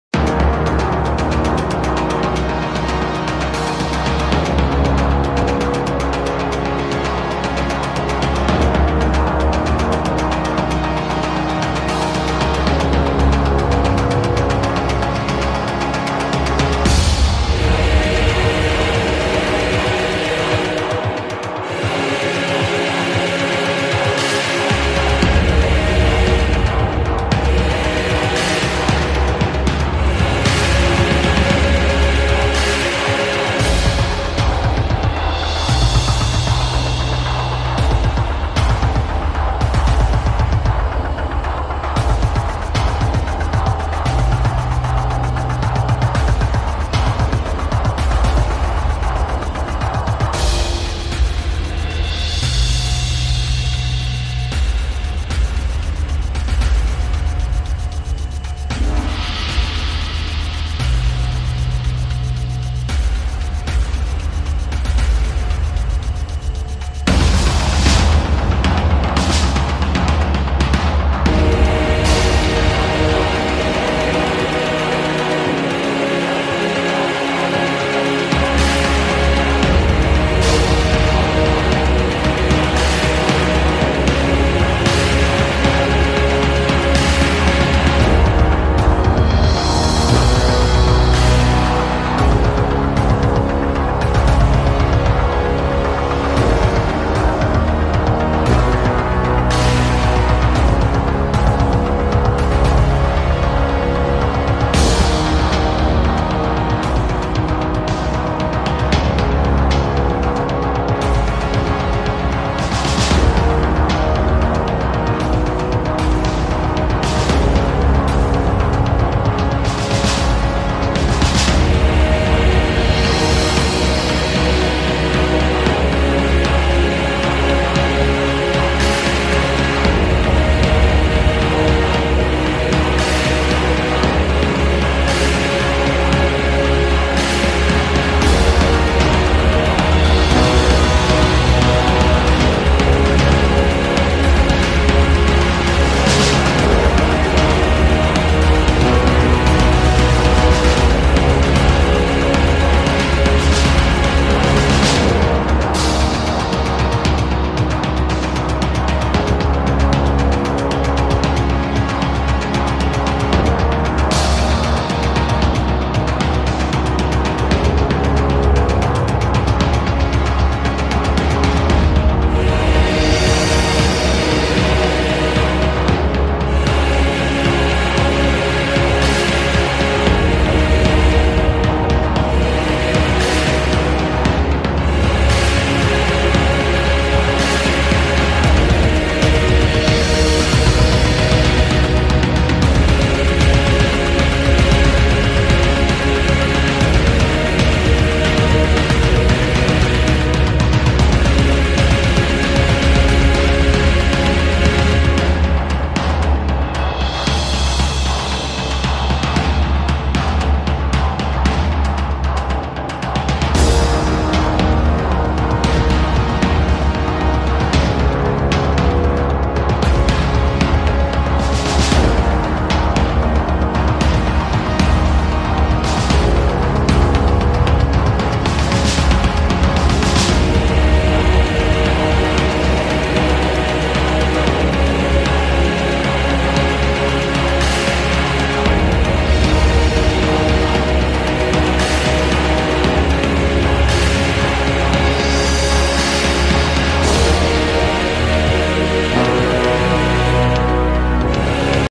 Giant American Bull Terrier | American bully puppies | Large dog breeds | American bully training
Giant and super most dangerous Amarican bully Pitbulls in the world  video compilation